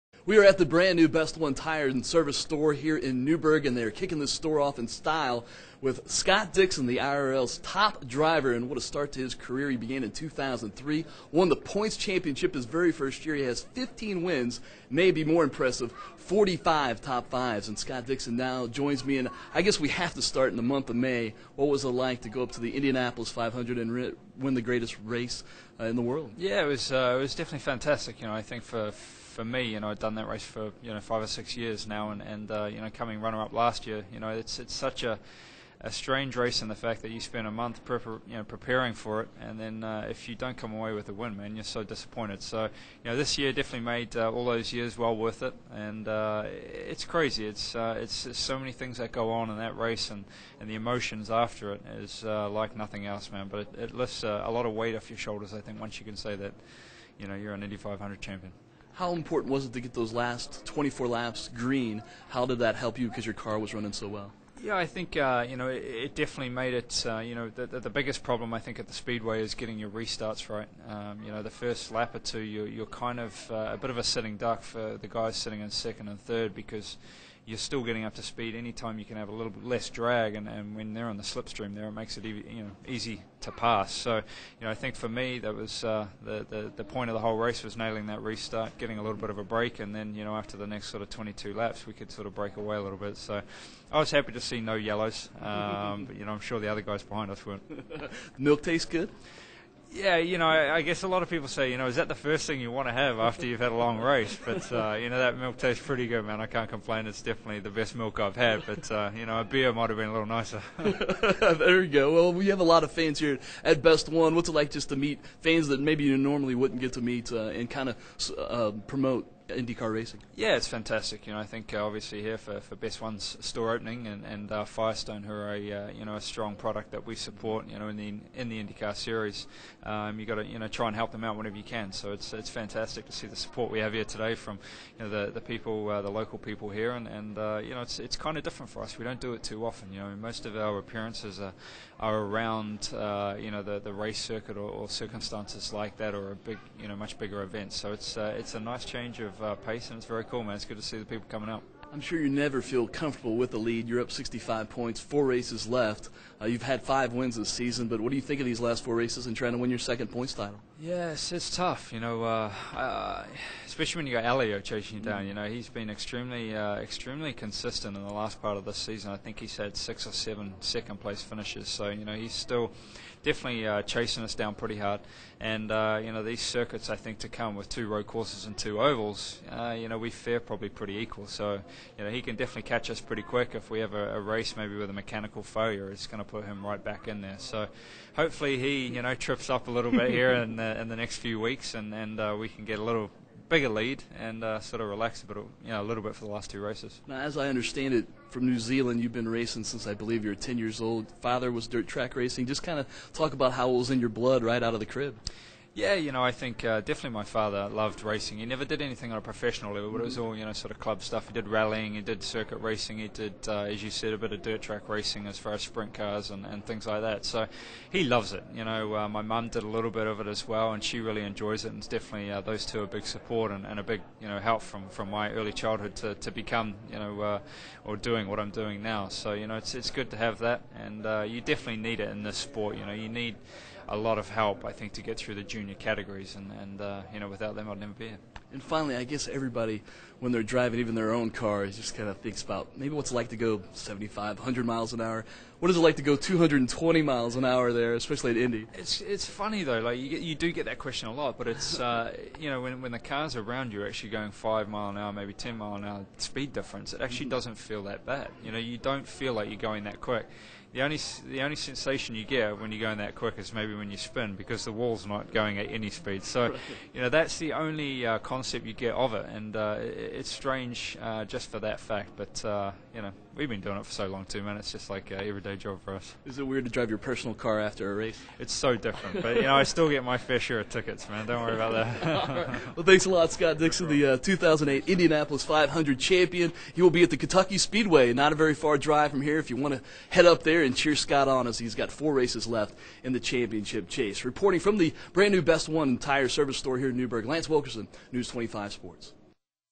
Interview with Indy 500 Champ Scott Dixon
Interview with 2008 Indianapolis 500 Champion Scott Dixon in Newburgh, IN on August 6, 2008.

Dixon was such a class act.  Great to talk with.  He is now my favorite driver!